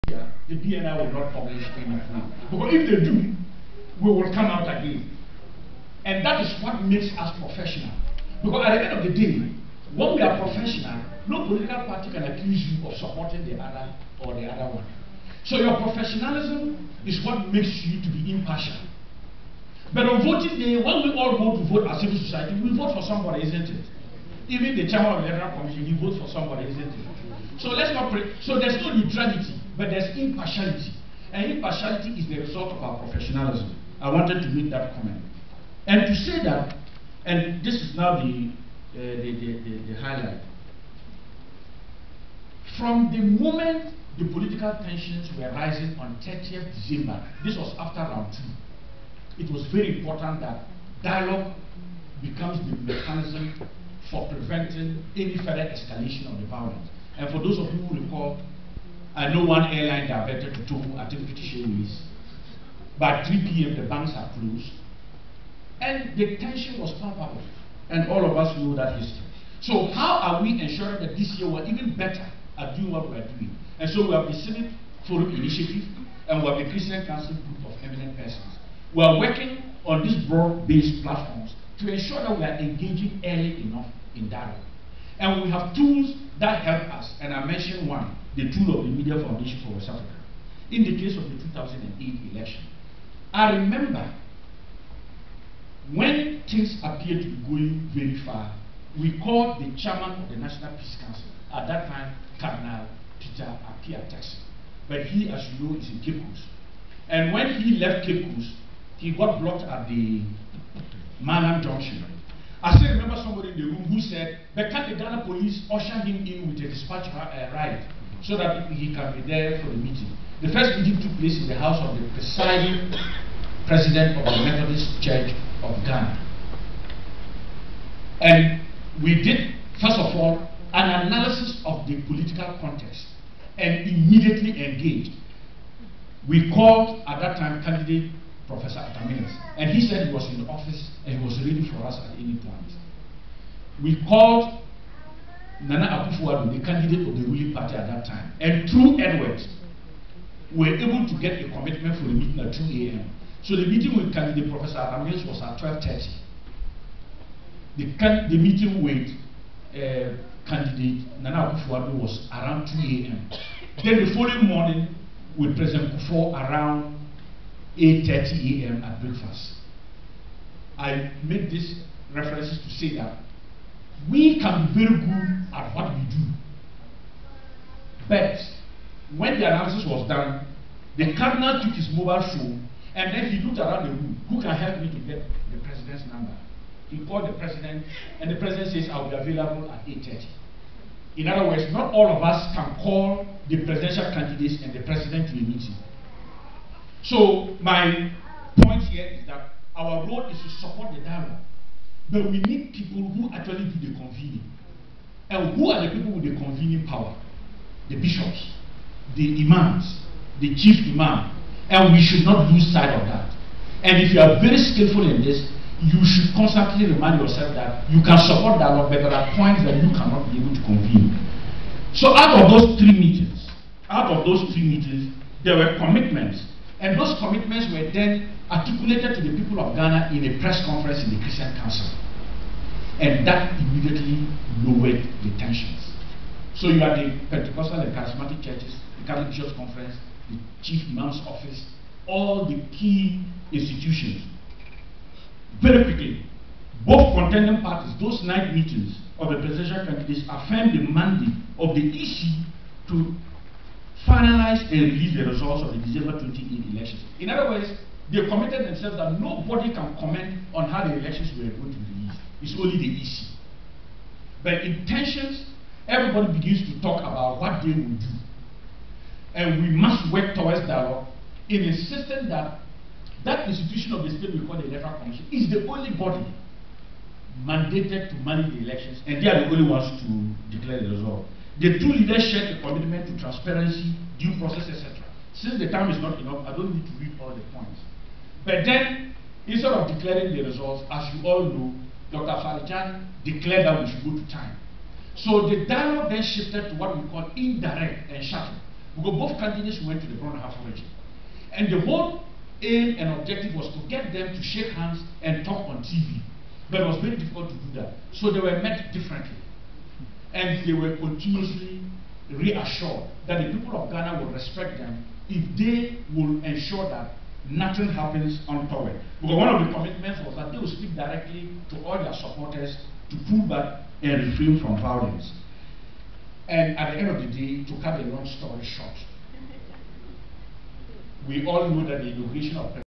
Mr. Emmanuel Bombande of WANEP-Ghana at the Star convention.AVI
Mr. Emmanuel Bombande of WANEP-Ghana speaking on conflict preventive reporting at the just ended STAR-Ghana semi-annual elections thematic convention